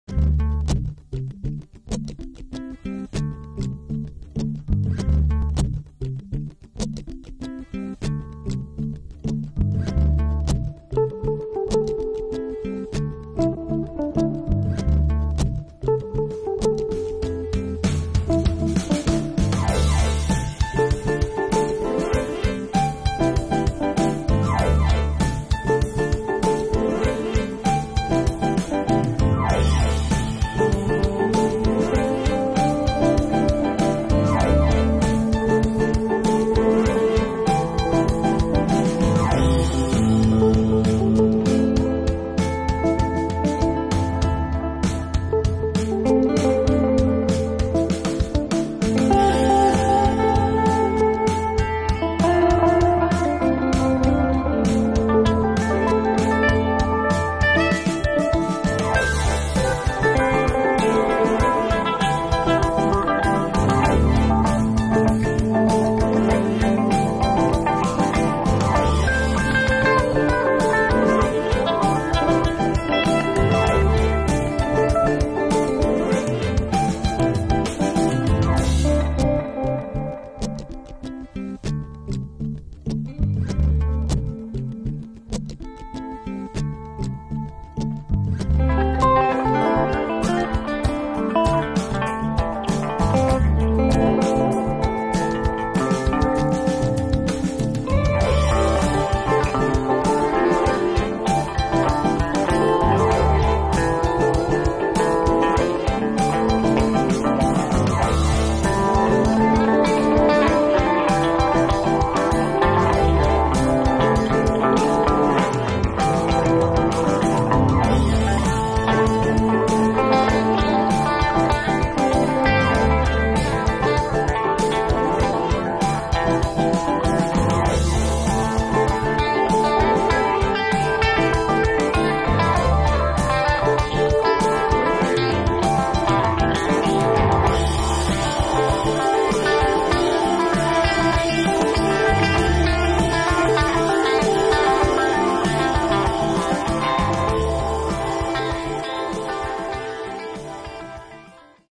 Painting a VW Bug Timelapse. Psychedelic Art Car
I was fortunate to get the opportunity to paint an old VW bug.